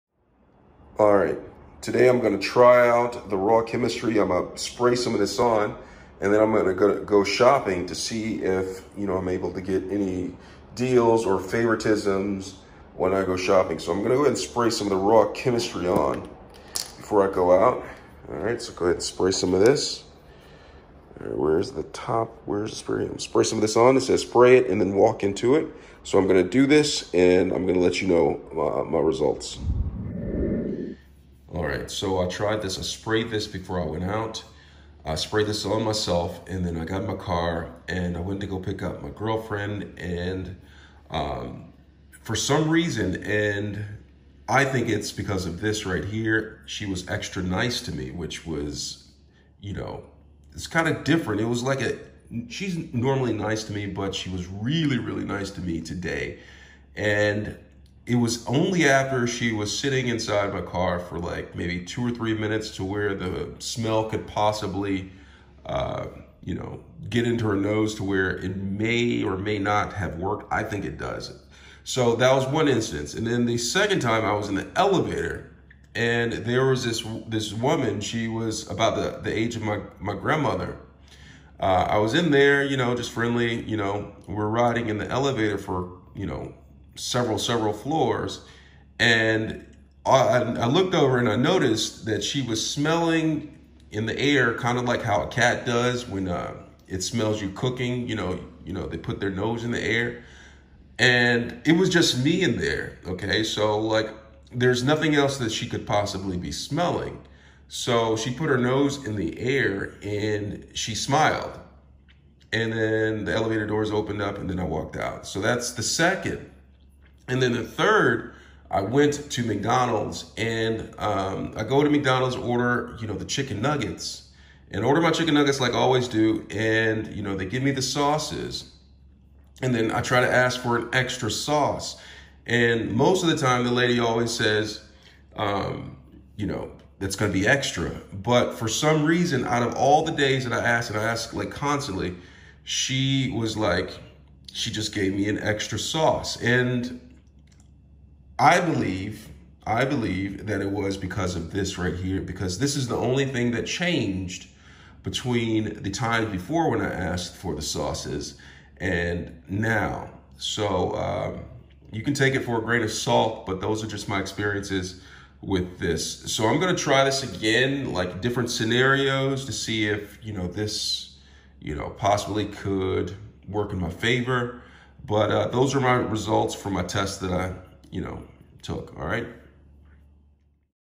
My RawChemistry Pheromone Cologne Experience
Discussing my Experience with Pheromone Cologne call  RawChemistry. This particular Pheromone Cologne is the Bold, Extra Strength Formula. Formulated to be enticing and attention grabbing. Made with our patented blend of scents and oils to entice and attract.